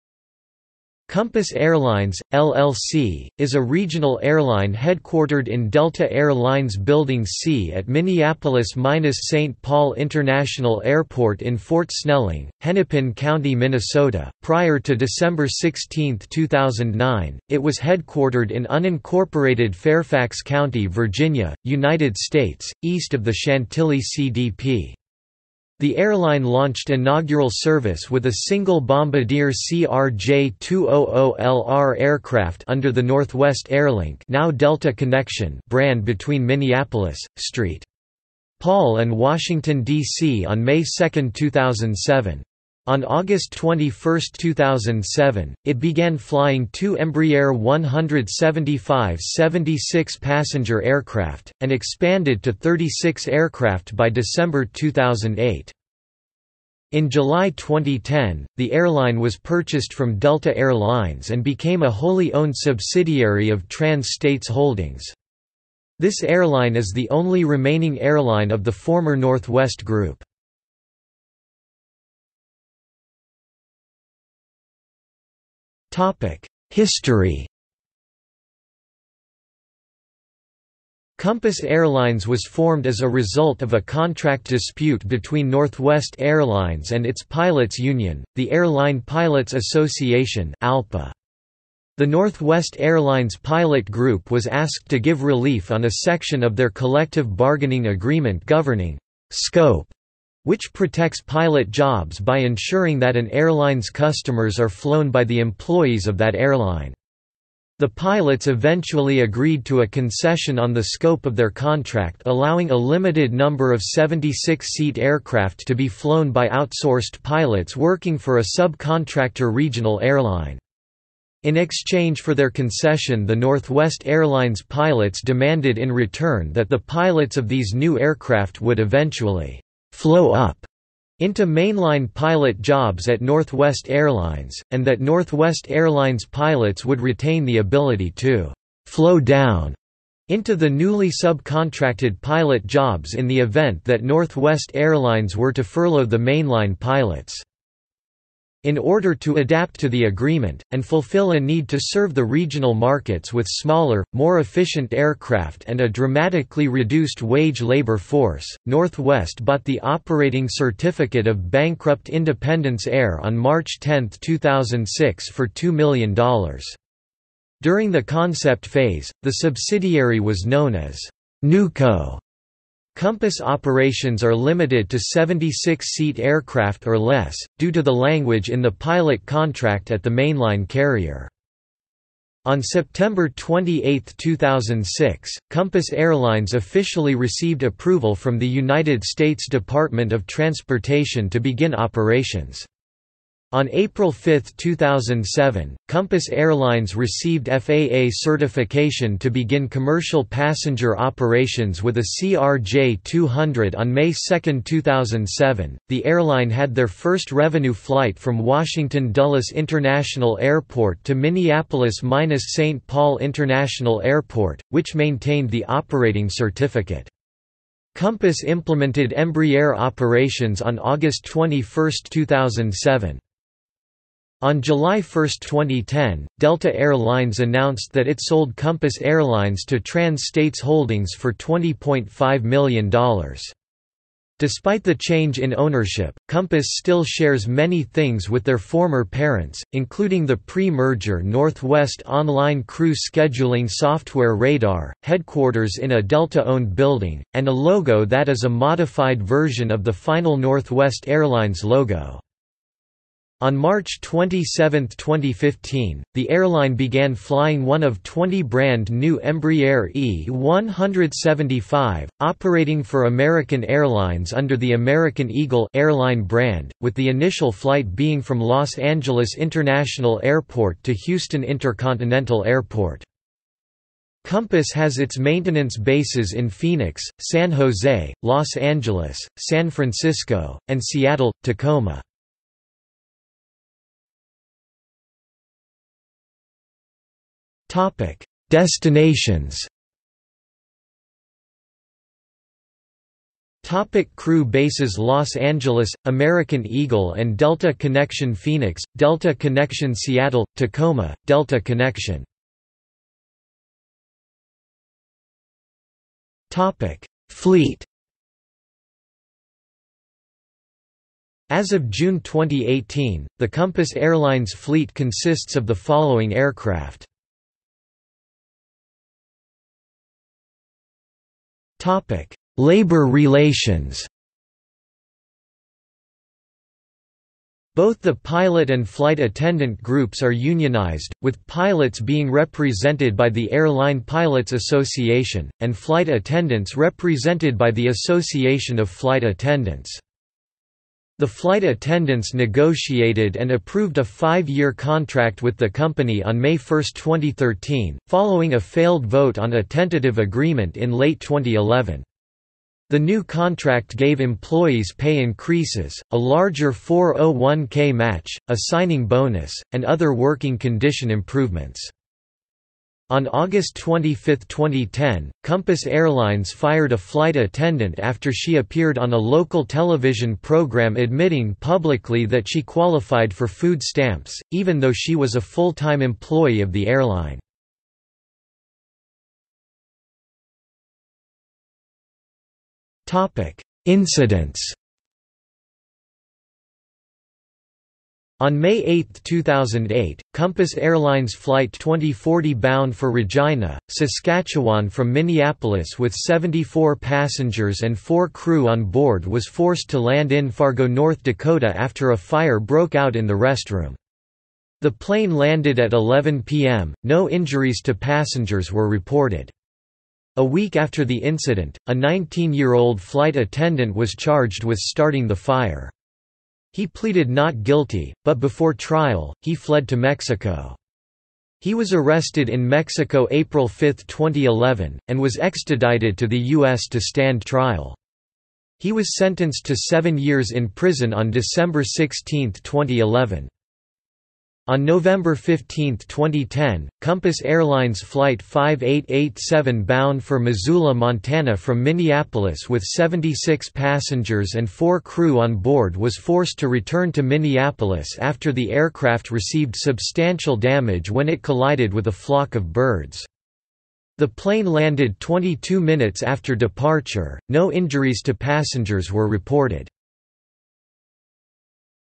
Compass Airlines | Wikipedia audio article
00:01:12 1 History
00:04:44 2 Destinations
00:04:53 3 Crew Bases
00:05:02 4 Fleet
00:05:18 5 Labor relations
00:06:19 6 Incidents

- Socrates

SUMMARY
Compass Airlines, LLC, is a regional airline headquartered in Delta Air Lines Building C at Minneapolis−Saint Paul International Airport in Fort Snelling, Hennepin County, Minnesota; prior to December 16, 2009, it was headquartered in unincorporated Fairfax County, Virginia, United States, east of the Chantilly CDP. The airline launched inaugural service with a single Bombardier CRJ200LR aircraft under the Northwest Airlink (now Delta Connection) brand between Minneapolis/St. Paul and Washington, D.C. on May 2, 2007. On August 21, 2007, it began flying two Embraer 175 76-passenger aircraft, and expanded to 36 aircraft by December 2008.
In July 2010, the airline was purchased from Delta Air Lines and became a wholly owned subsidiary of Trans States Holdings. This airline is the only remaining airline of the former Northwest Group.